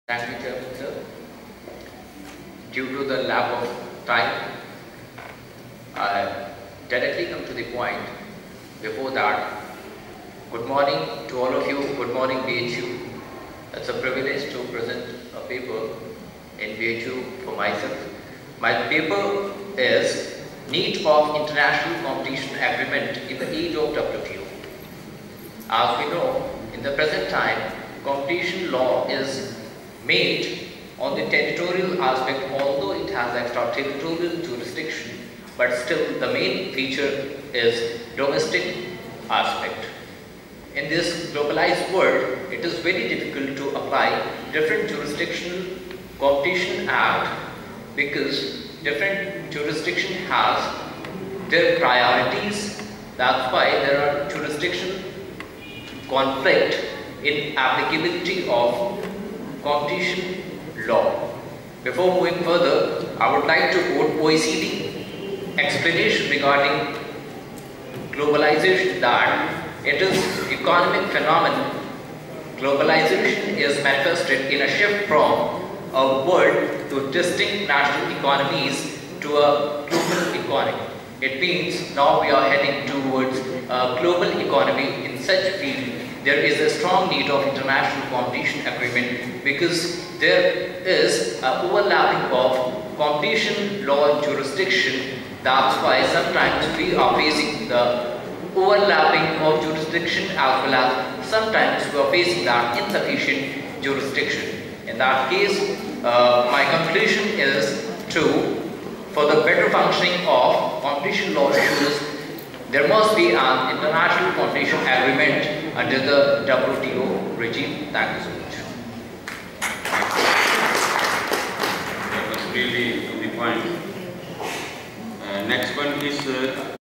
Mohammad Atif Khan, Lecturer, Amity Law School, at International Conference on Trade Law
Paper presentation on International Competition Agreement at International Conference on International Trade Law